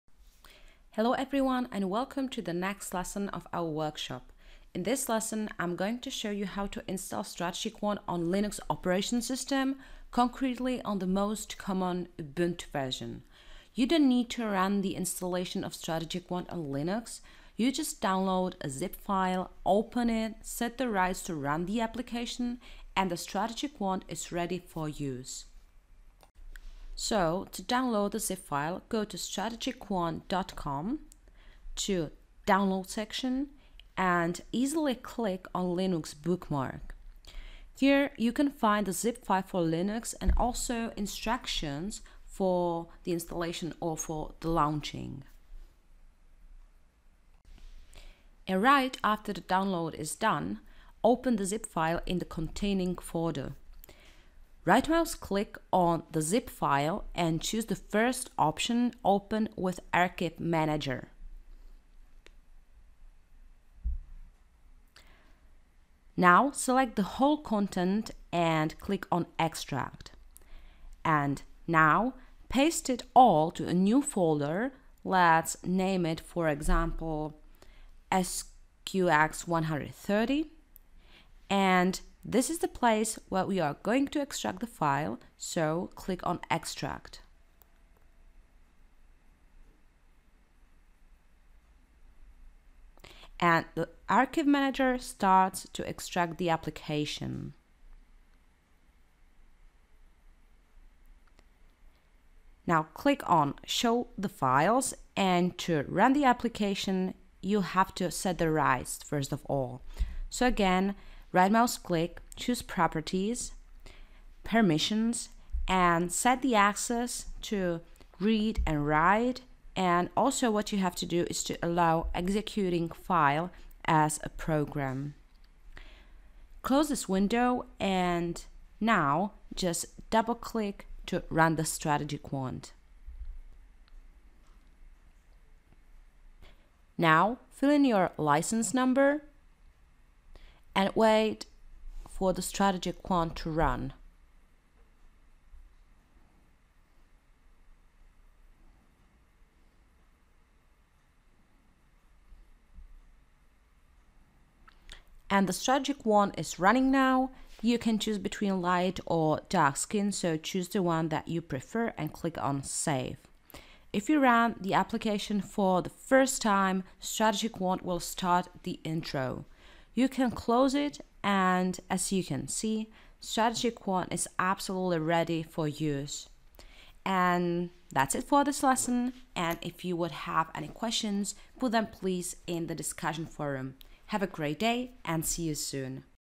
How to install StrategyQuant X on Linux
In this video, you will find instructions on how to install the StrategyQuant X on Linux.

Our other tools for traders:
QuantMonitor - monitor your trading platform